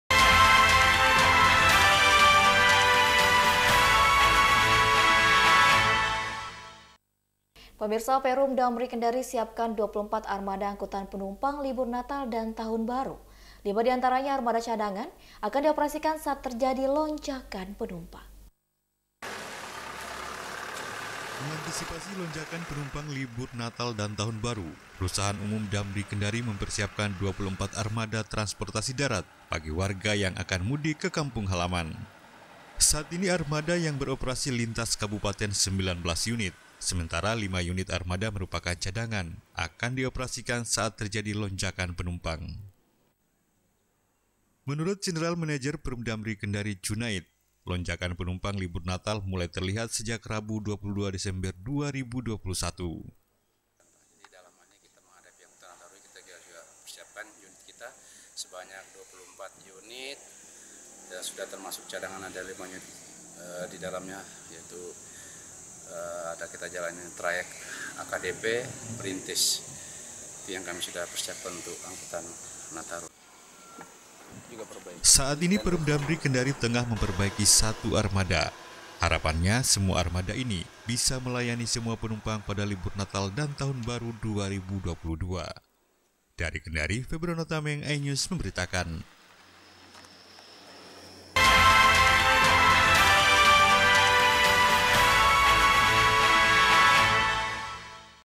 24 Armada Bus Damri Kendari Disiapkan untuk Melayani Warga Saat Libur Nataru
iNews Sultra, 23 Desember 2021